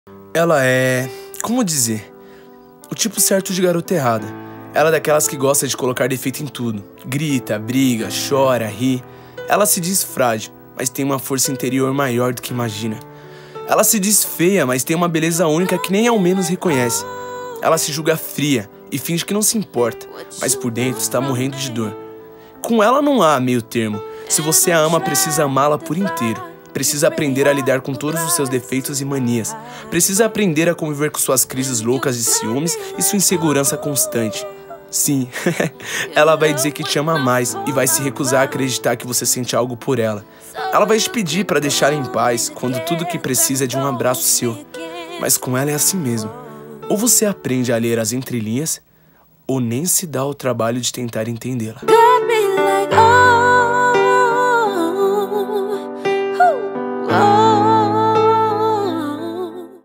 ela é o TIPO CERTO DE GAROTA ERRADA [CITAÇÃO]
TEXTO ORIGINAL:
Ela é… como dizer? O tipo certo de garota errada. É daquelas que gosta de colocar defeito em tudo. Grita, briga, chora, ri. Ela se diz frágil, mas tem uma força interior maior do que imagina. Ela se diz feia, mas tem uma beleza única que nem ao menos reconhece. Ela se julga fria, e finge que não se importa, mas por dentro está morrendo de dor. Com ela, não há meio termo. Se você a ama, precisa amá-la por inteiro. Precisa aprender a lidar com todos os seus defeitos e manias. Precisa aprender a conviver com suas crises loucas de ciúmes, e sua insegurança constante. Sim, ela vai dizer que te ama mais, e recusar-se a acreditar que você sente algo por ela. Ela vai te pedir para deixá-la em paz, quando tudo que precisa é de um abraço seu. Mas com ela é assim mesmo: ou você aprende a ler as entrelinhas, ou nem se dá ao trabalho de tentar